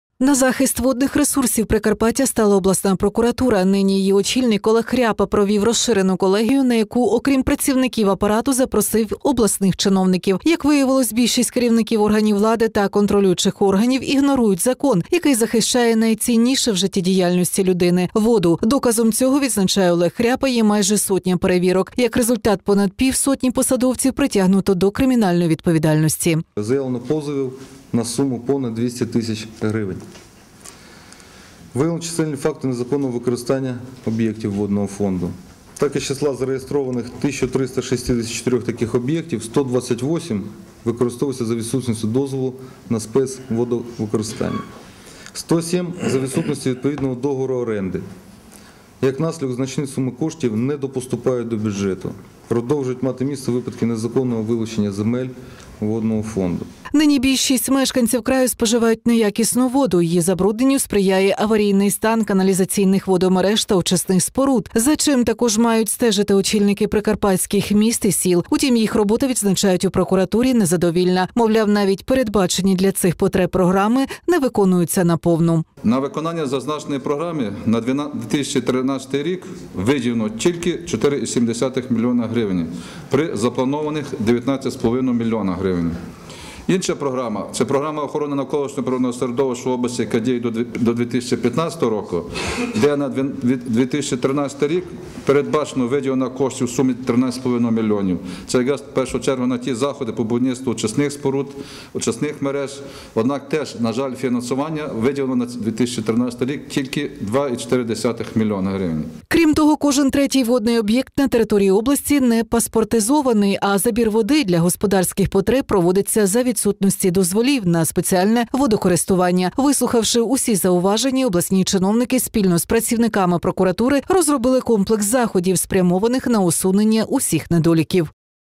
На захист водних ресурсів Прикарпаття стала обласна прокуратура
На захист водних ресурсів прикарпаття стала обласна прокуратура.  Нині її очільник Олег Хряпа  провів розширену колегію на яку окрім працівників апарату запросив й обласних чиновників. Канал 402